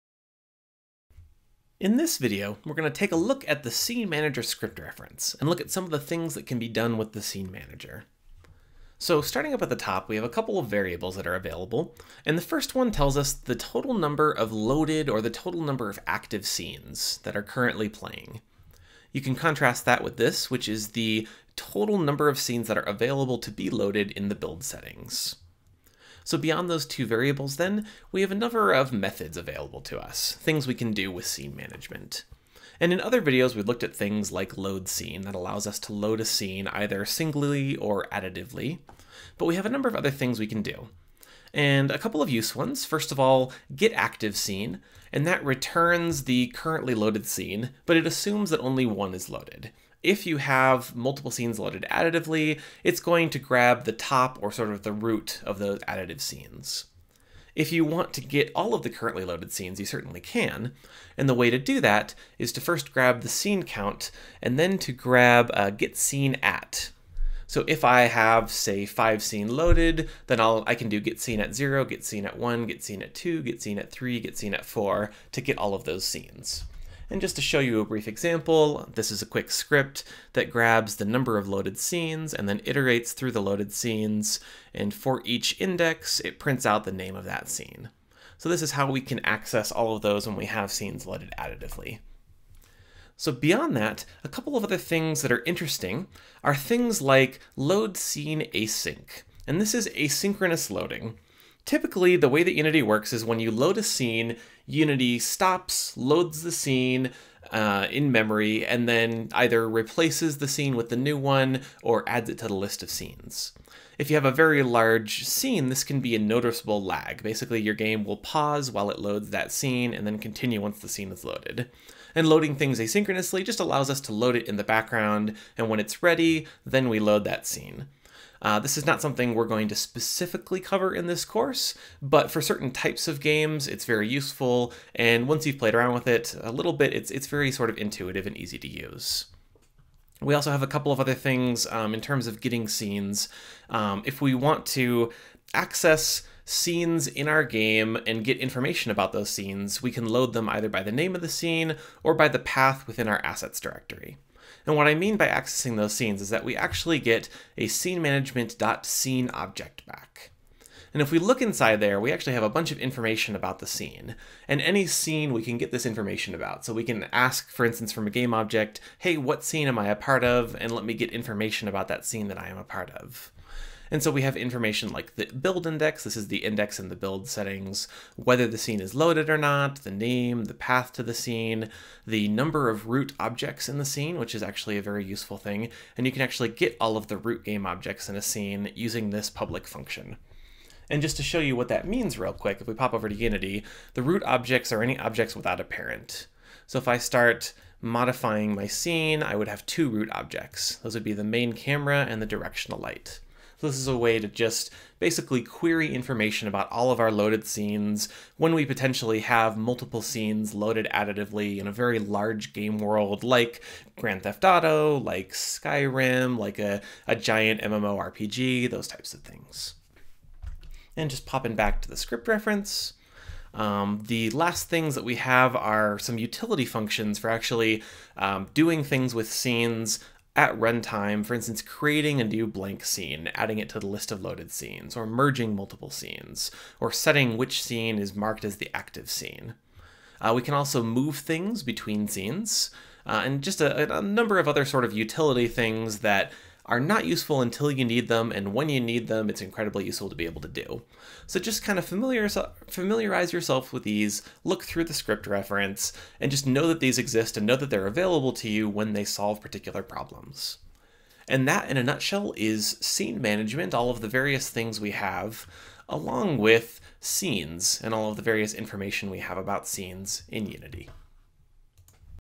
Unity Scenes 4: What Can You Do with SceneManager?
A look at the various utilities that come with the SceneManager and SceneManager.Scene classes in Unity and how to call these from code.